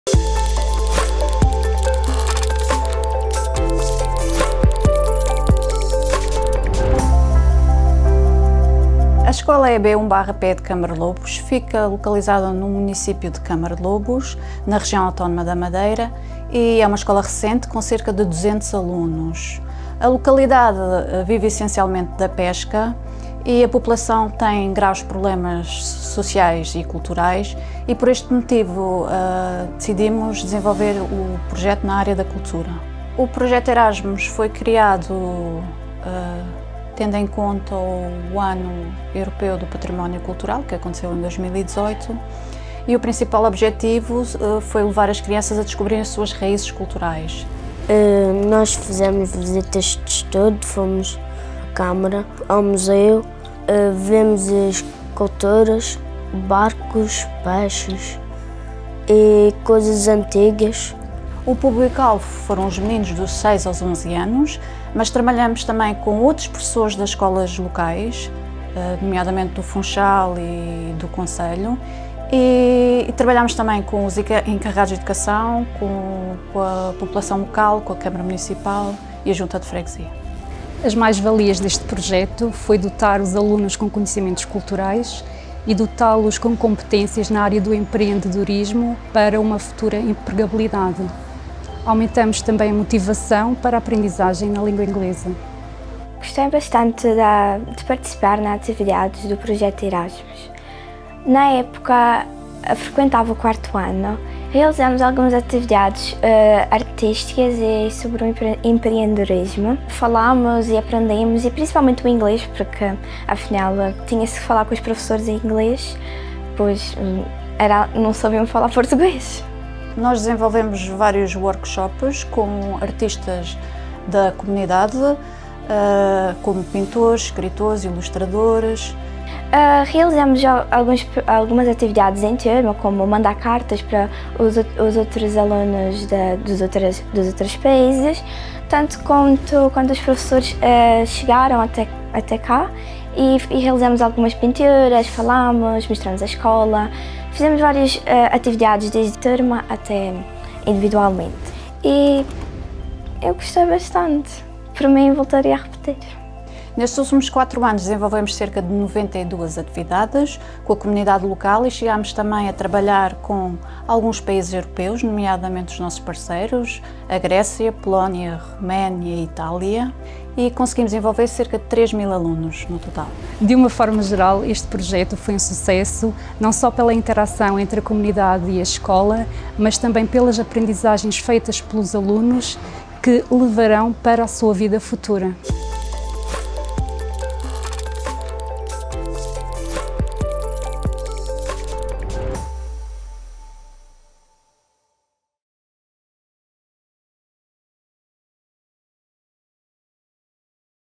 Boa Prática 2022 | EB1/PE de Câmara de Lobos
Boas Práticas 2022

Projeto Erasmus+ Educação e Formação
Ensino Escolar

EB1/PE de Câmara de Lobos
"Mult'Culture - Promote Education For Cultural Heritage"
Coordenadoras: Alexandra Ferreira e Margarida Mota